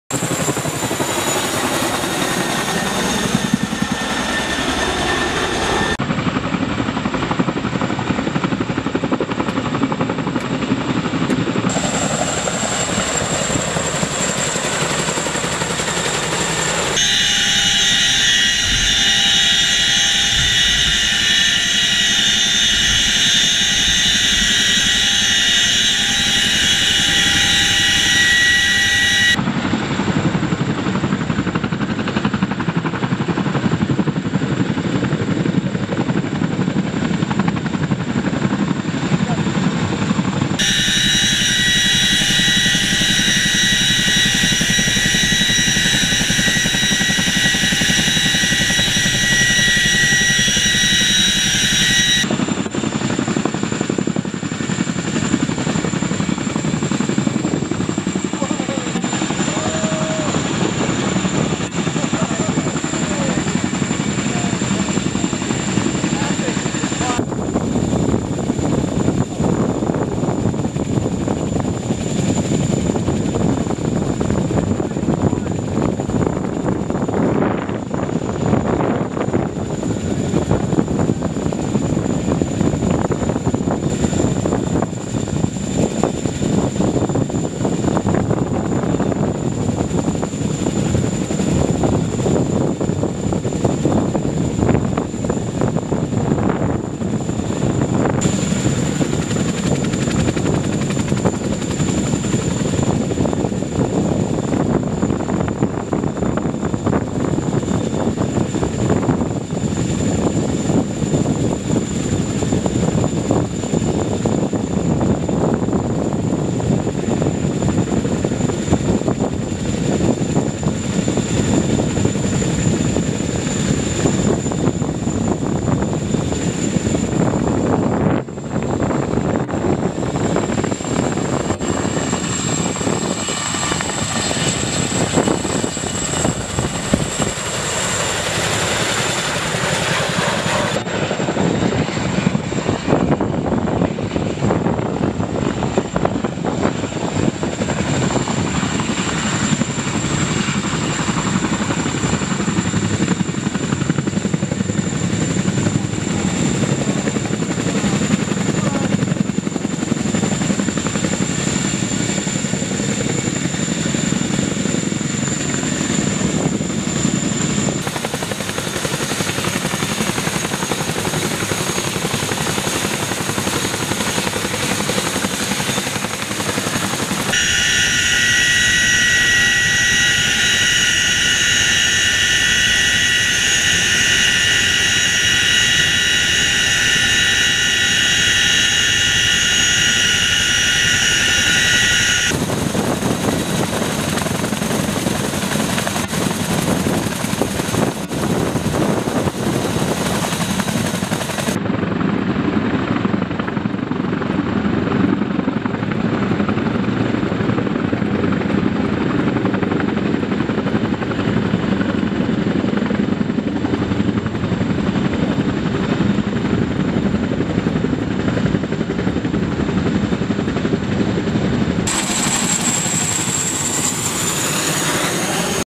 Washington ARNG CH-47 Chinook
The Washington Air National Guard Army Aviation Support Facility #1's CH-47 "Chinook" helicopter performed a HELOCAST at American Lake. The crew provided multiple turns, and performed a caving ladder and boat drop.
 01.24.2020
Video by Sara Morris
Joint Forces Headquarters, Washington National Guard